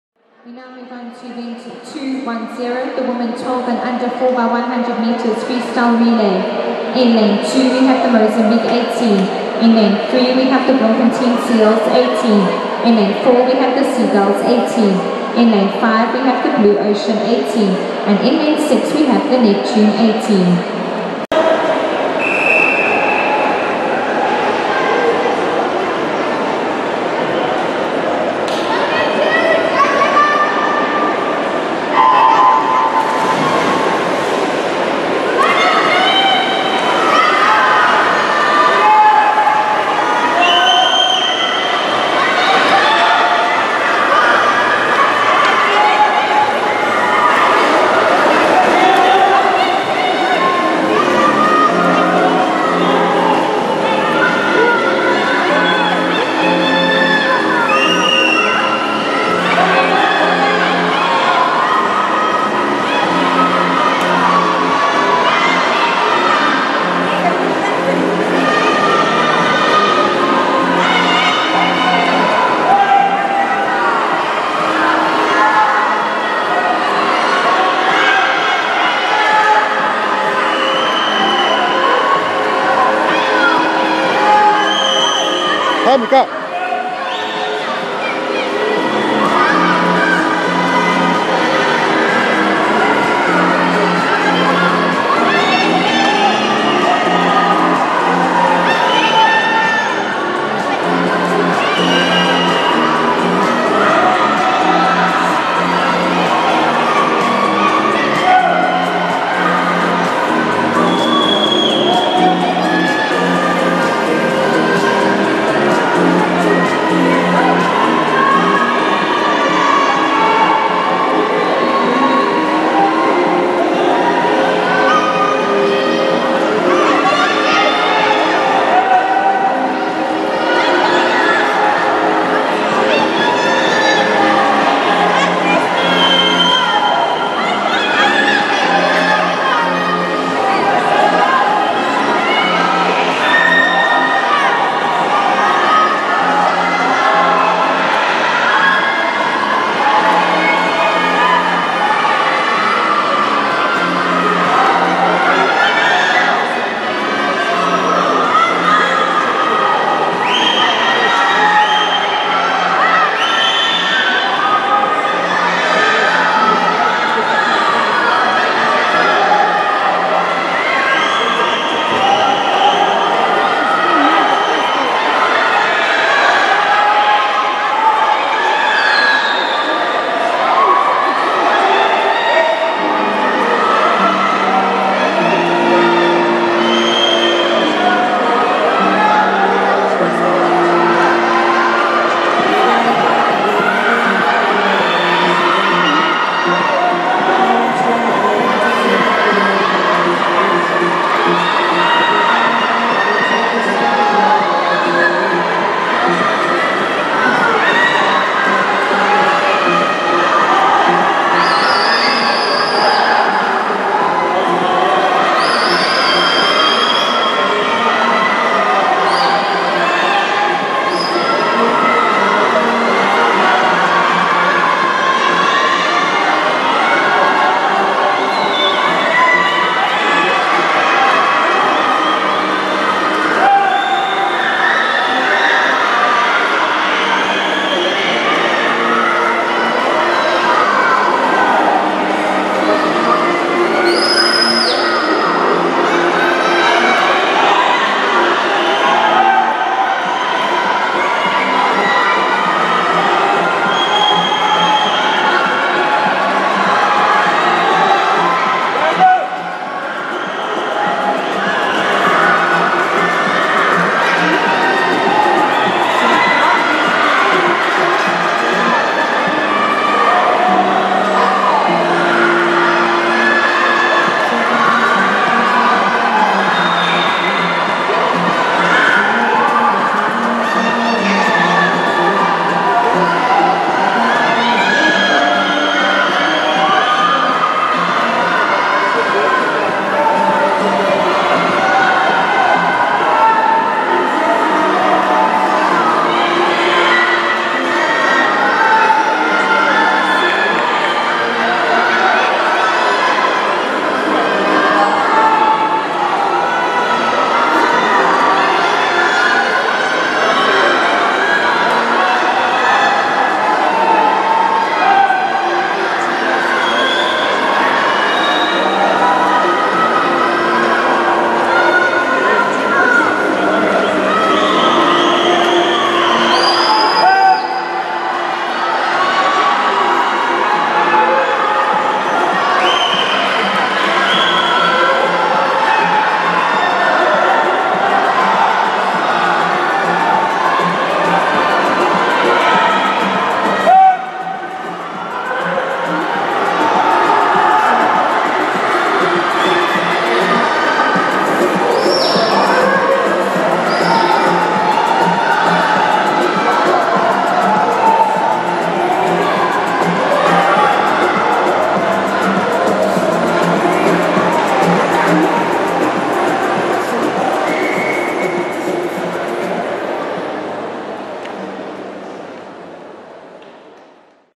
SSA L1 Champs 2016 Women 12 & U 4 x 100 LC m free relay
Gold medal winning team Seagulls at the SSA L1 swimming championships 2016.  What a race!!  Monica Botha, aged 9 swam a Personal Best in the first leg of 1:16:56.  This time is not acknowledged but we know now that she can do this. The team consisted of one 9-year-old, 2 x 11-year-old and a 12-year-old swimmer.